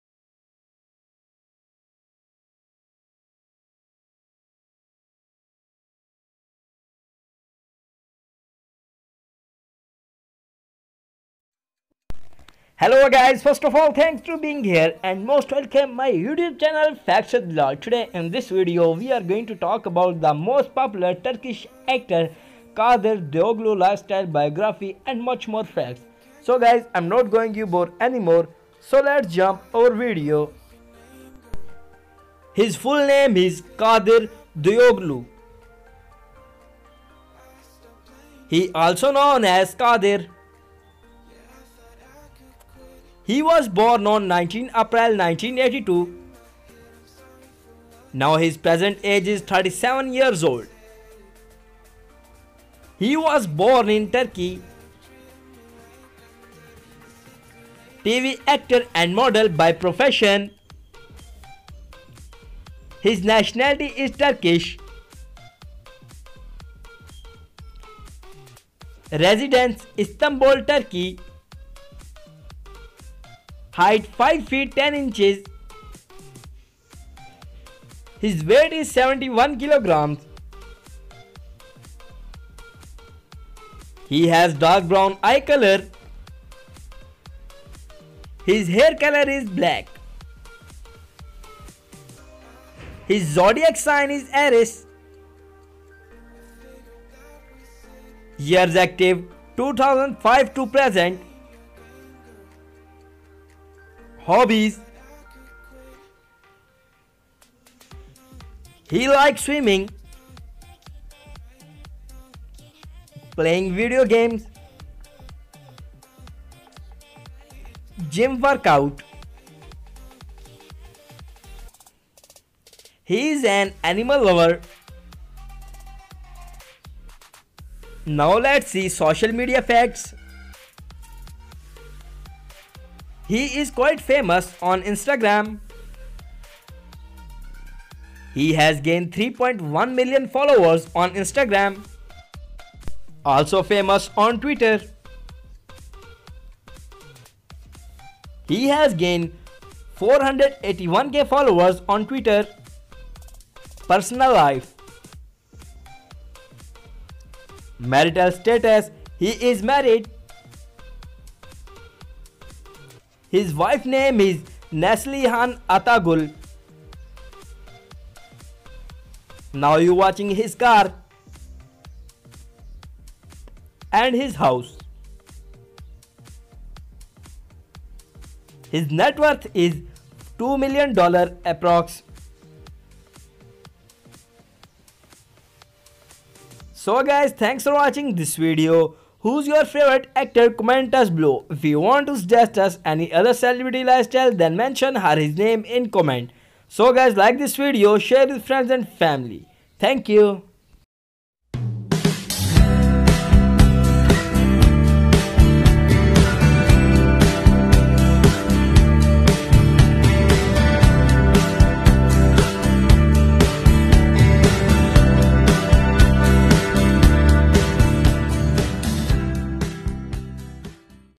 Kadir Dogulu (Wife: Neslihan Atagul) | Lifestyle| Networth | Age | FactsWithBilal
The Most popular and Most Talented Turkish  Actor and Model Kadir Dogulu Biography & Top 10 Facts 2020

This Information Collected by Google!